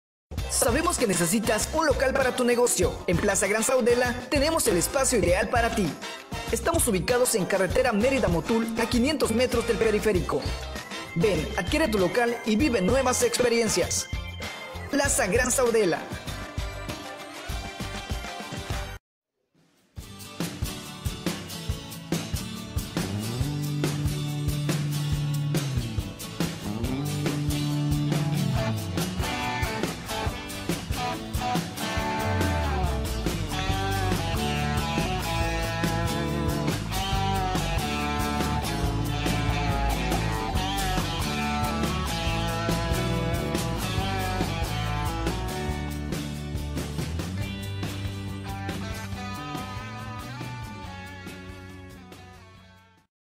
LOCALES EN RENTA PLAZA SAUDELA MERIDA YUCATAN
DESCRIPCIÓN
Plaza Gran Saudela Locales comerciales en renta sobre carretera Mérida-Conkal (Motul) a 10 minutos de plaza Altabrisa.

Excelente flujo vehicular frente a la plaza. Cuenta con restaurantes ancla como Las Hijas de las Tostadas y Bem Brasileiro

DISPONIBILIDAD Locales disponibles en

Edificio A: -A9 (8x8 m) 64 m2 más terraza $8,000 + IVA **500 pesos de mantenimiento, adicionales al precio de renta.

Locales disponibles en Edificio Central: -B24 (8x8 m) 64 m2 $8,000 + IVA -B25 y B26 (8x8 m) 64 m2 más terraza $8,000 + IVA

**500 pesos de mantenimiento, adicionales al precio de renta. OTRAS

CARACTERÍSTICAS -

Altura de la Planta Baja: 4.25 mts. -Capacidad para mezanine. -

Altura Planta Alta: 3.45 mts -Se entrega en obra gris con preparaciones hidráulicas, sanitarias y eléctricas.

-Cuota de mantenimiento $500
70 cajones de estacionamiento
1 mes de gracia -
Se requiere seguro de responsabilidad civil